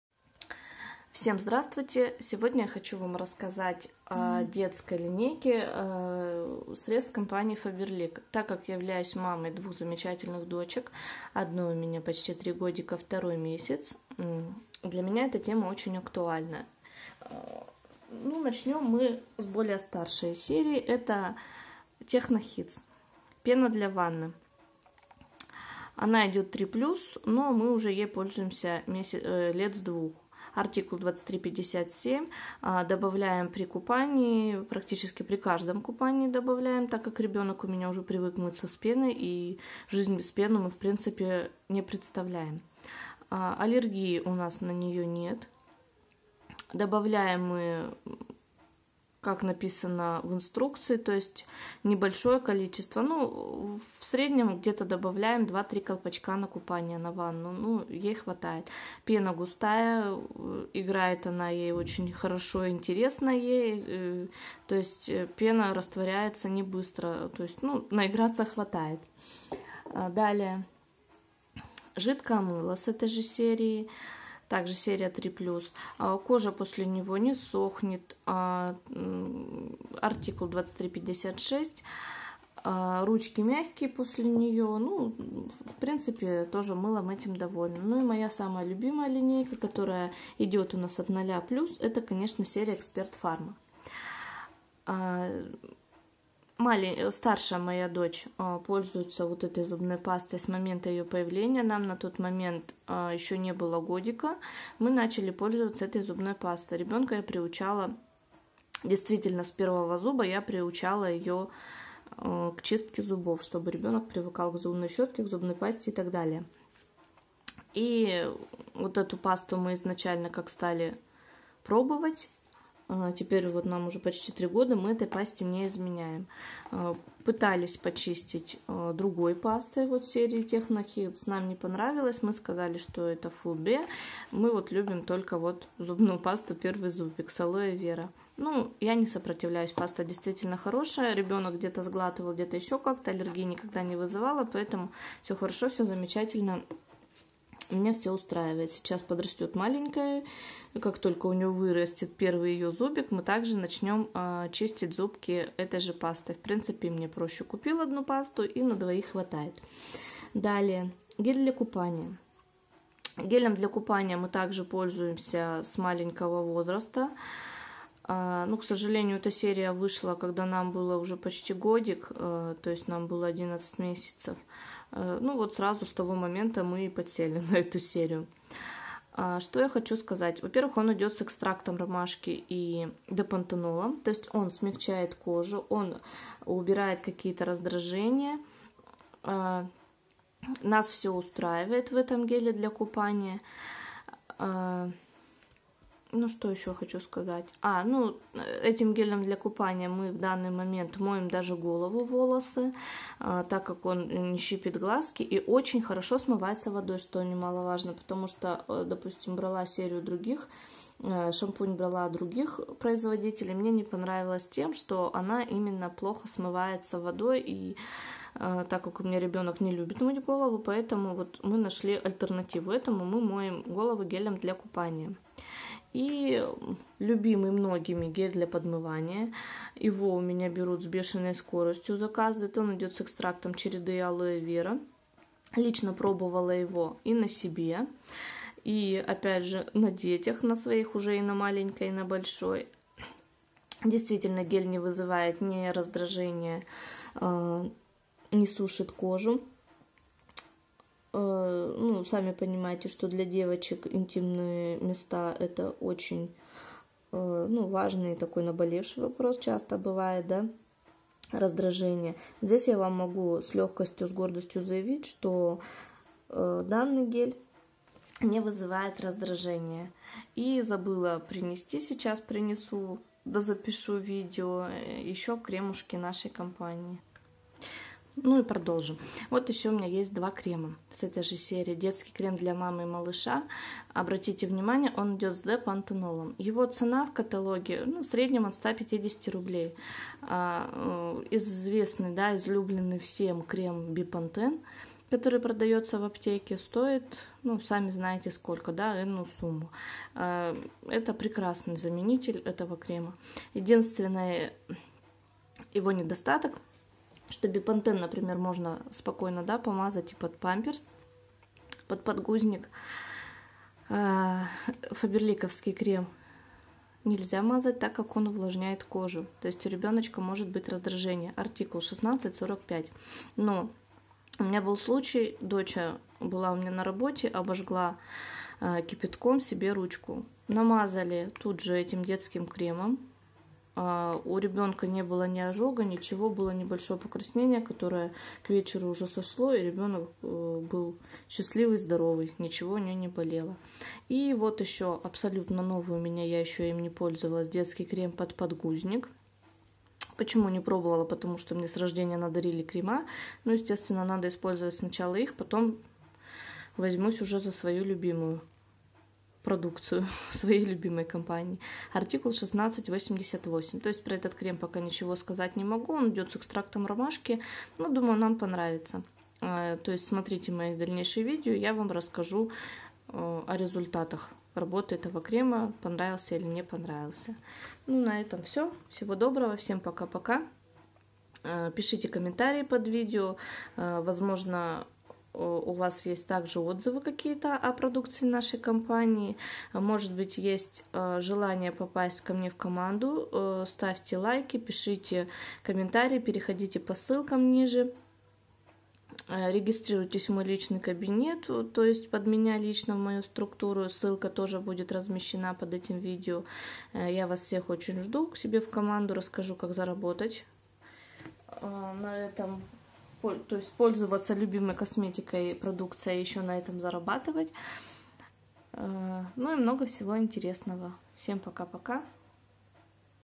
Забота с первых дней жизни
Пена для ванны Techno Kids благодаря своей плотности и стойкости превращает купание ребёнка в творческий процесс.

Серия для детей Expert Pharma:
Зубная паста для первых зубов (0+) с алоэ вера.

Гель для купания с экстрактом ромашки и Д-пантенолом бережно ухаживает за нежной кожей и волосами малыша. Формула без слез. 0+

Гель для подмывания с экстрактом череды и алоэ вера идеально подходит для интимной гигиены малыша. Мягкая pH формула. 0+